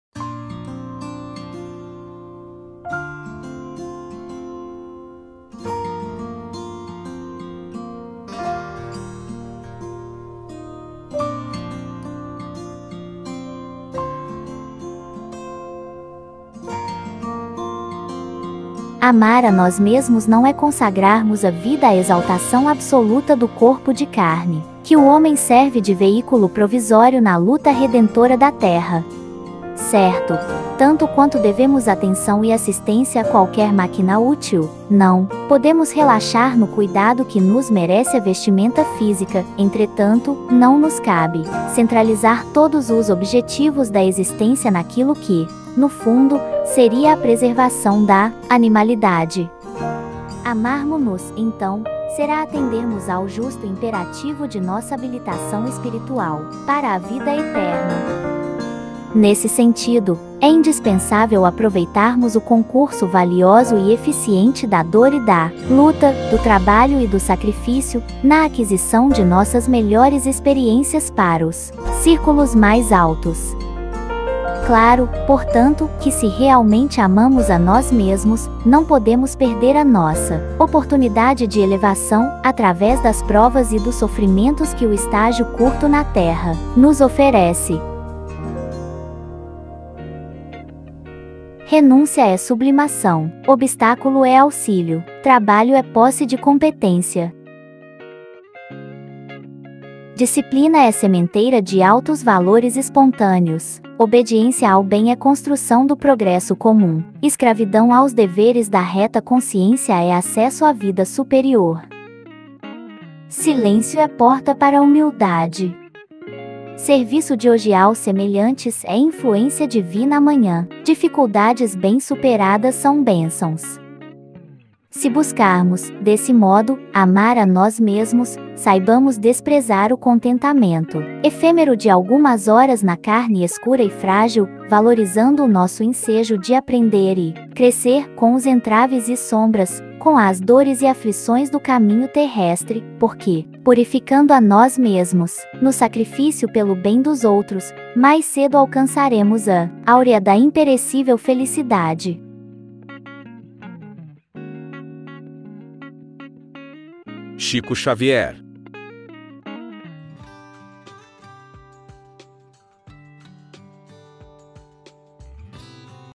Amar a Nós Mesmos
Emmanuel por Chico Xavier
livro Construção do Amor
Amar a nós mesmos não é consagrarmos a vida à exaltação absoluta do corpo de carne
que o homem serve de veículo provisório na luta redentora da Terra.
Certo, tanto quanto devemos atenção e assistência a qualquer máquina útil, não
podemos relaxar no cuidado que nos merece a vestimenta física, entretanto, não nos cabe
centralizar todos os objetivos da existência naquilo que, no fundo, seria a preservação da
animalidade.
Amarmo-nos, então, será atendermos ao justo imperativo de nossa habilitação espiritual
para a vida eterna.
Nesse sentido, é indispensável aproveitarmos o concurso valioso e eficiente da dor e da
luta, do trabalho e do sacrifício, na aquisição de nossas melhores experiências para os
círculos mais altos.
A pedra que fugisse ao buril e o vaso que se desviasse do clima asfixiante do forno
jamais seriam arrancados do primitivismo agreste aos espetáculos da beleza e da utilidade.
Claro, portanto, que se realmente amamos a nós mesmos, não podemos perder a nossa
oportunidade de elevação, através das provas e dos sofrimentos que o estágio curto na Terra
nos oferece.
Renúncia é sublimação.
Obstáculo é auxílio.
Trabalho é posse de competência.
Disciplina é sementeira de altos valores espontâneos.
Obediência ao bem é construção do progresso comum.
Escravidão aos deveres da reta consciência é acesso à Vida Superior.
Silêncio é porta para a humildade.
Serviço de hoje aos semelhantes é influência divina amanhã.
Dificuldades bem superadas são bênçãos.
Se buscarmos, desse modo, amar a nós mesmos, saibamos desprezar o contentamento
efêmero de algumas horas na carne escura e frágil, valorizando o nosso ensejo de aprender e
crescer, com os entraves e sombras, com as dores e aflições do caminho terrestre, porque,
purificando a nós mesmos, no sacrifício pelo bem dos outros, mais cedo alcançaremos a
áurea da imperecível felicidade. Emmanuel por Chico Xavier do livro